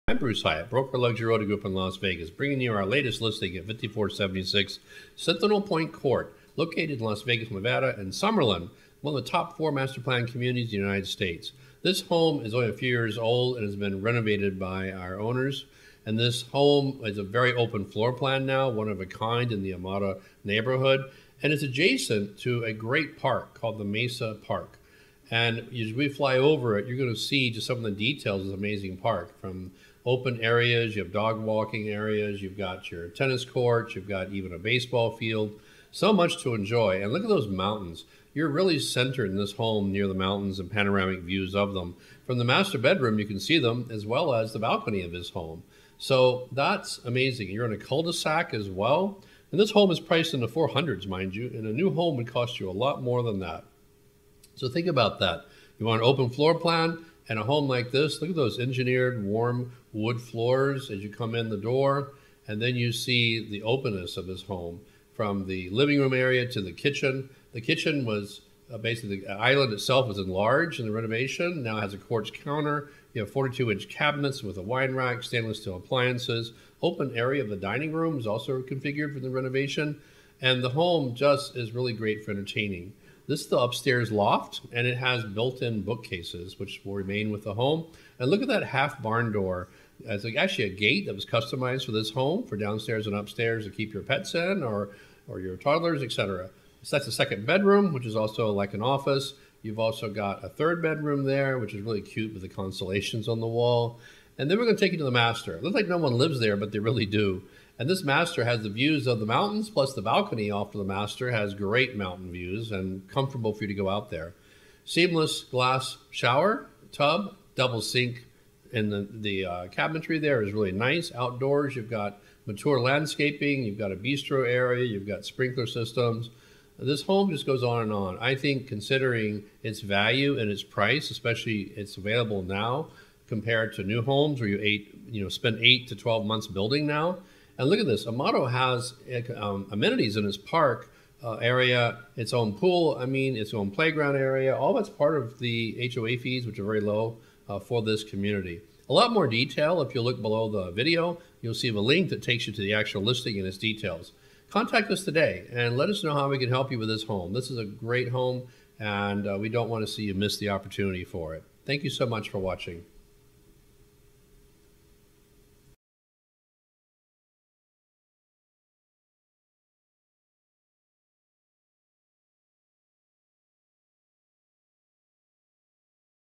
5476 Sentinel Point Ct Las Vegas Home for Sale
Luxury Realty Group in Las Vegas presents its latest listing with this Summerlin, open floor plan, 2 story home.  It has 3 bedrooms, 2.5 baths, 2245 sq feet and is on a cul-de-sac lot adjacent to a great park.  Priced in the $400's; compare this renovated home to new construction that would cost significantly more.